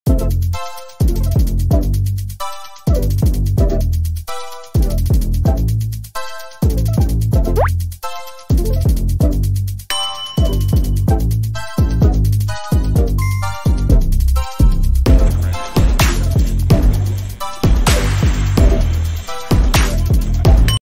Lego Stop motion Contest! (open)
Hello there this lego stop motion contest is currently open to everyone
also with Prizes!
Participate Now!